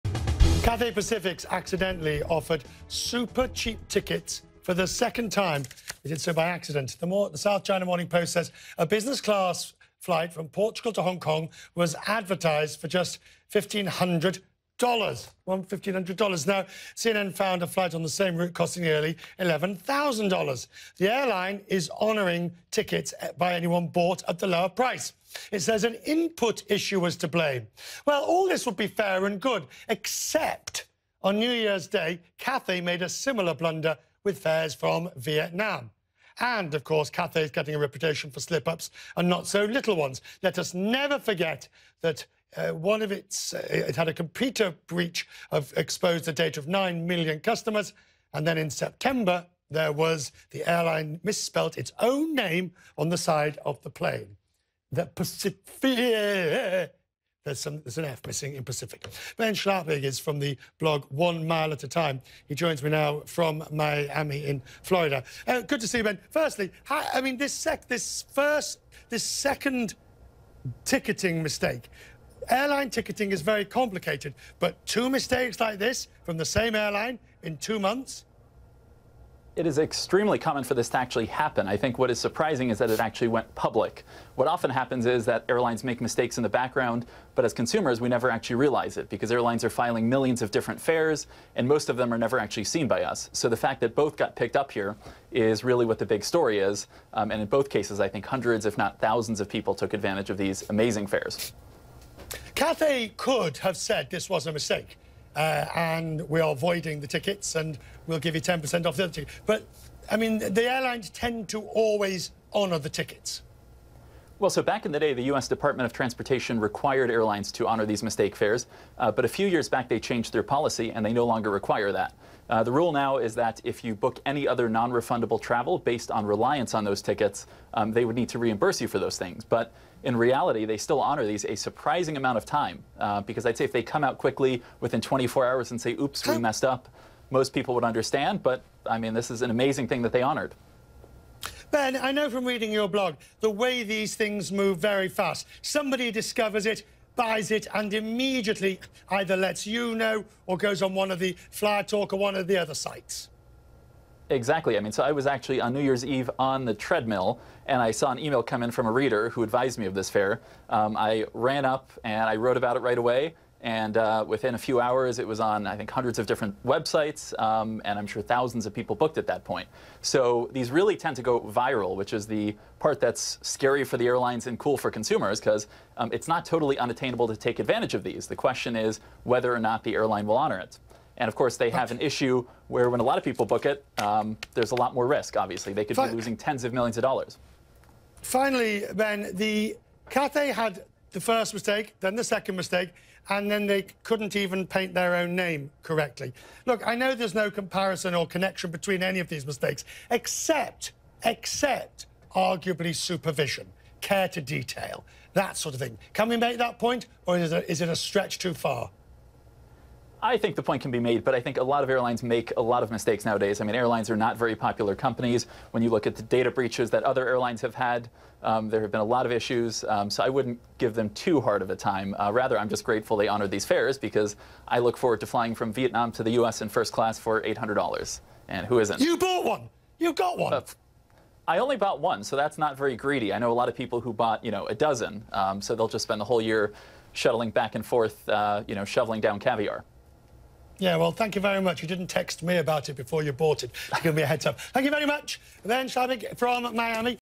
Cathay Pacific in new fare mix-up
Cathay Pacific accidentally offered super-cheap tickets for the second time. Plus, the airline had a data breach and misspelled its own name on the side of a plane. One Day at a Time's Ben Schlappig analyzes the mix-ups.